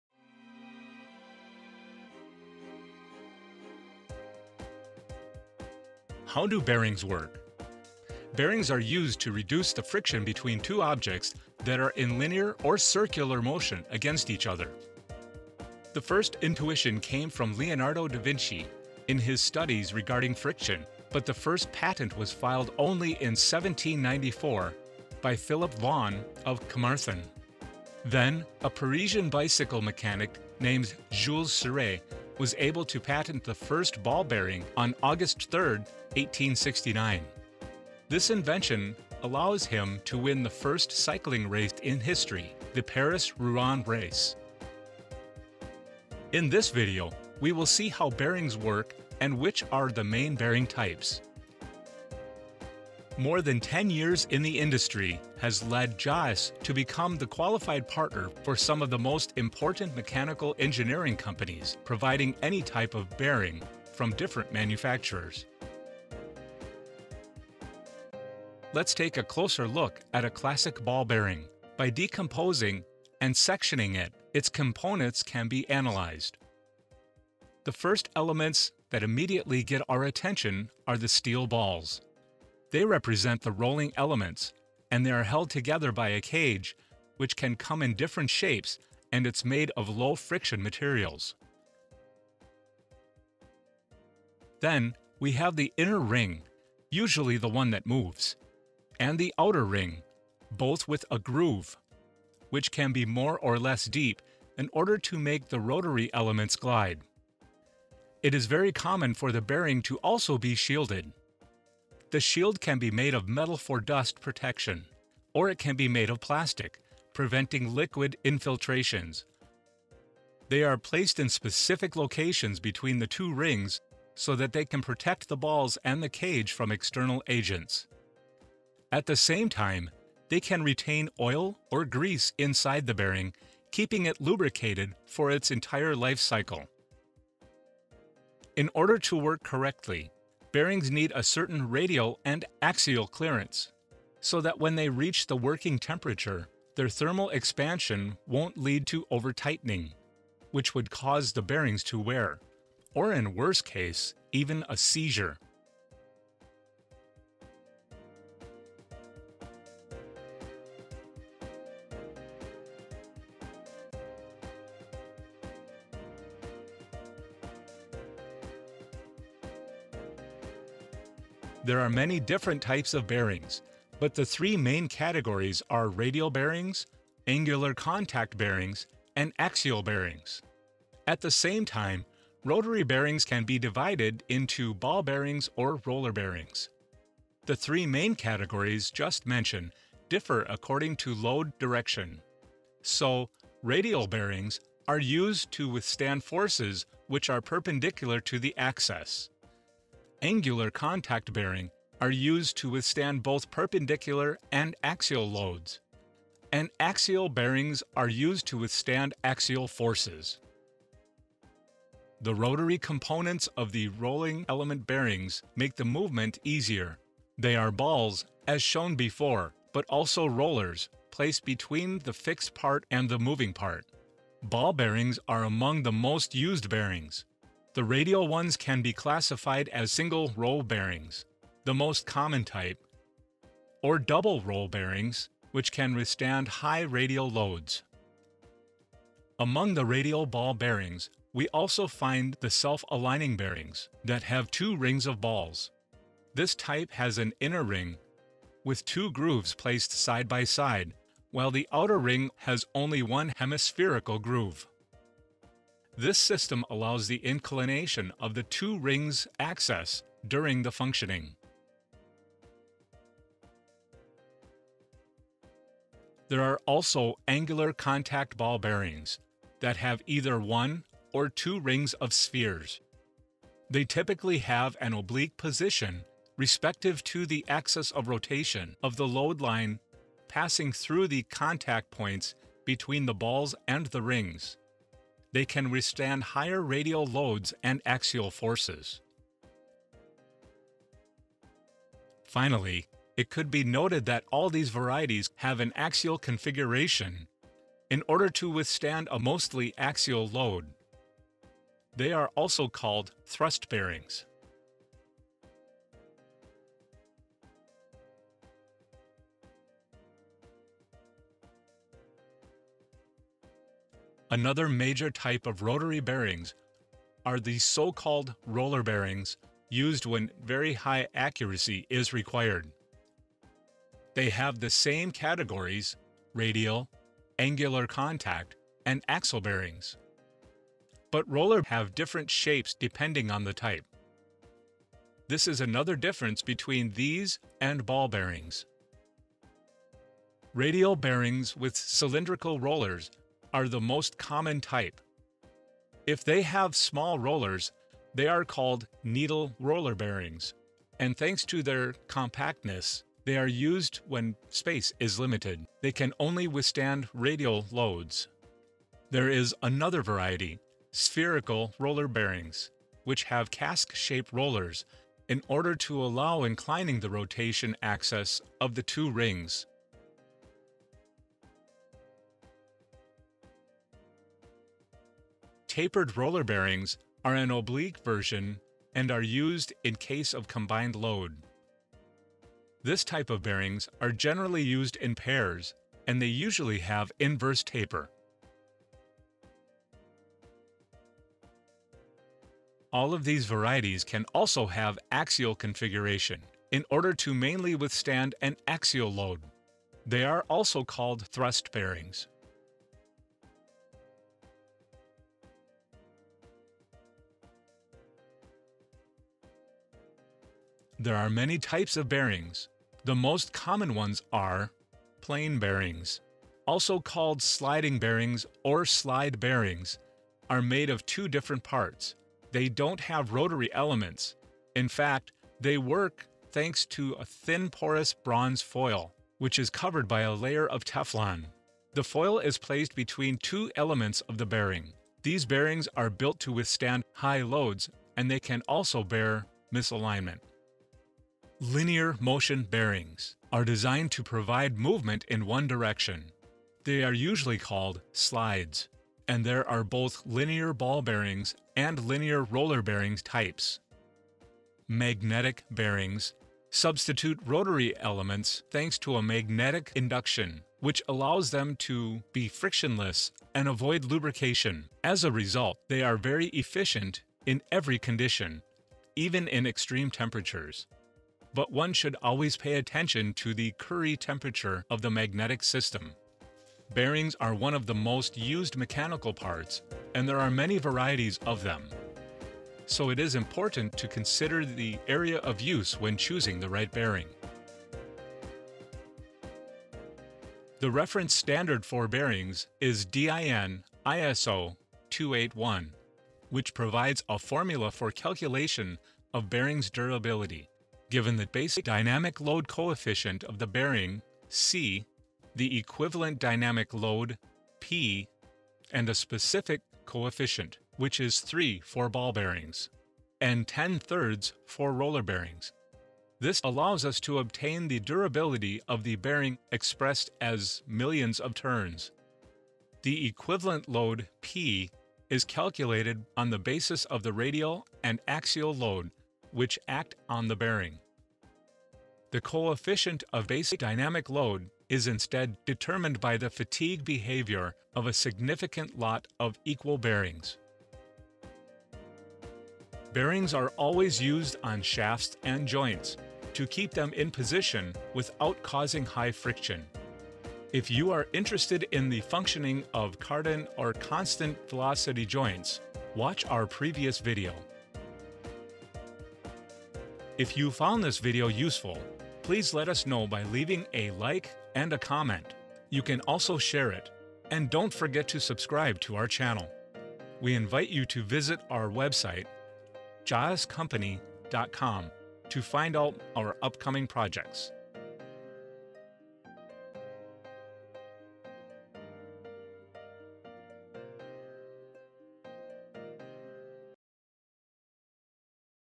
How do ball and roller bearings work? Types and durability calculation. DIN ISO 281
Bearings are used to reduce the friction between two objects that are in linear or circular motion against each other.

The first intuition came from Leonardo Da Vinci in his studies regarding friction.
But the first patent was filed only in 1794 by Philip Vaughan of Carmarthen.
Then, a Parisian bicycle mechanic named Jules Suriray, was able to patent the first ball bearing on August 3rd 1869. This invention allows him to win the first cycling race in history: the Paris-Rouen race.

Now let’s take a closer look at a classical ball bearing. By decomposing and sectioning it, its  components can be analysed.
The first elements that immediately get our attention are the steel balls.
They represent the rolling elements, and they’re held together by a cage which can come in different shapes and it’s made of low friction materials.
Then, we have the inner ring (usually the one that moves) and the outer ring, both with a groove, which can be more or less deep, in order to make the rotary elements glide.
It is very common for the bearing to also be shielded. The shield can be made of metal for dust protection, or it can be made of plastic, preventing liquid infiltrations. They are placed in specific locations between the two rings, so that they can protect the balls and the cage from external agents. At the same time they can retain oil or grease inside the bearing, keeping it lubricated for its entire life cycle.
In order to work correctly, bearings need a certain radial and axial clearance, so that when they reach the working temperature, their thermal expansion won’t lead to overtightening (which would cause the bearing’s wear), or in the worst case, even a seizure!

Ball bearings are among the most used bearings. The radial ones can be classified as single-row bearings (the most common type) or double-row bearings (which can withstand high radial loads).
Among the radial ball bearings we also find the self-aligning bearings, that have two rings of balls. This type has an inner ring with two grooves placed side by side, while the outer ring has only one hemispherical groove. This system allows the inclination of the two rings’ axis during the functioning.
There are also angular contact ball bearings, that have either one or two rings of spheres. They typically have an oblique position (respective to the axis of rotation) of the load line passing through the contact points between the balls and the rings. They can withstand higher radial loads and axial forces.
Finally, it should be noted that all these varieties can also have an axial configuration, in order to withstand a mostly axial load. They are also called thrust bearings.

The other major type of rotary bearings are the so called roller bearings, used when very high accuracy is required.  They have the same categories: radial, angular contact and axial bearings. But roller bearings have different shapes depending on the type: this is another difference between these and ball bearings.

Radial bearings with cylindrical rollers are the most common type. If they have small rollers, they’re called needle roller bearings, and thanks to their compactness they are used when space is limited. They can only withstand radial loads.
There is another variety: spherical roller bearings, which have cask-shaped rollers, in order to allow inclining the rotation axis of the two rings.
Tapered roller bearings are an oblique version and are used in case of combined load. This type of bearings are generally used in pairs and they usually have inverse taper.
All these variations can also have an axial configuration, in order to mainly withstand an axial load. They are also called thrust bearings.

Bearings are one of the most used mechanical parts and there are many varieties of them. So it is important to consider the area of use when choosing the right bearing.
The reference standard for bearings is DIN  ISO 281, which provides a formula for the calculation of bearings’ durability: given the dynamic load coefficient of the bearing “C”, the equivalent dynamic load “P” and a specific coefficient, which is 3 for ball bearings, and ten thirds for roller bearings. The “n” allows us to obtain the durability of the bearing expressed as millions of turns.